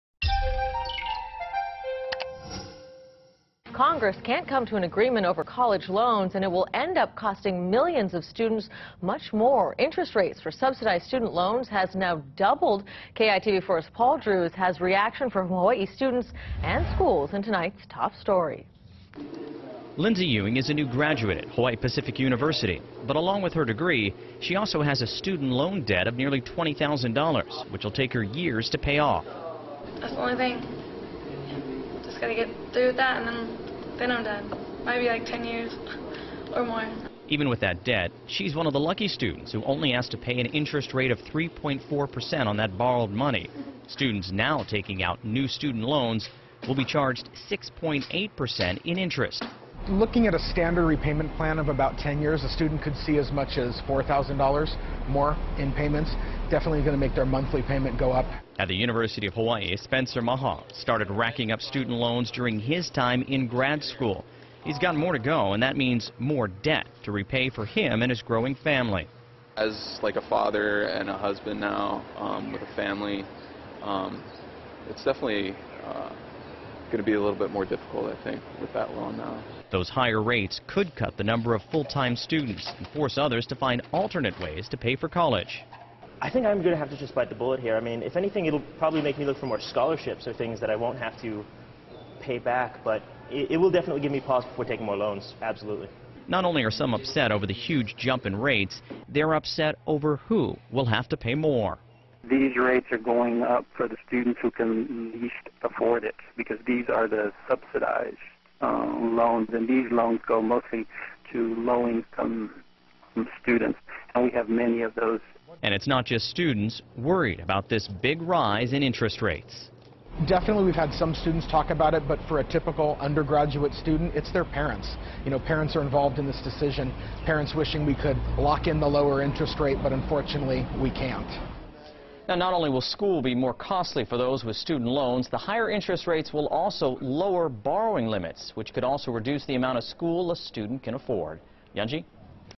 Student loan interest rates double
Congress can't come to an agreement over college loans and it will end up costing millions of student much more. For news you need to know,